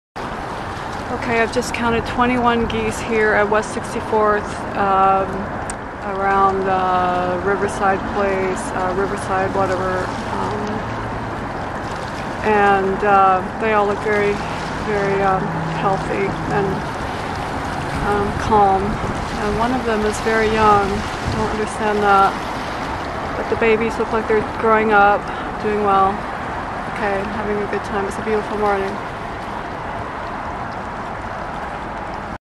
Hudson River geese on a beautiful early summer morning
The calm before the storm, young families hanging out on the shores of the Hudson River as the USDA fires us its killing machine. USDA derives the bulk of its "revenue" from killing contracts. It is diabolical what this agency is going to do these geese.

What a terrible lesson to set for children: kill freely.